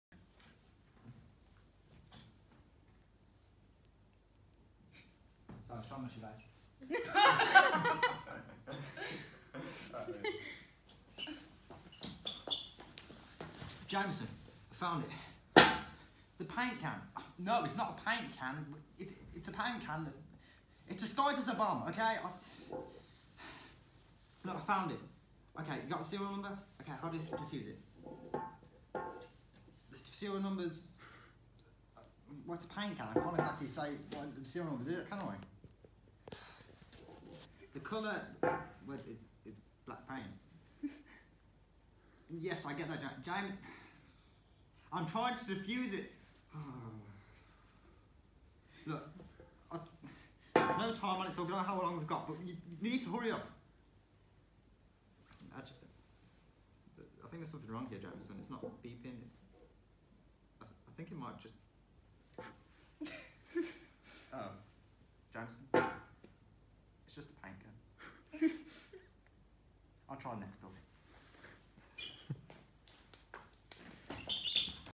Props Louie
Students are give a prop to create a short improvised scene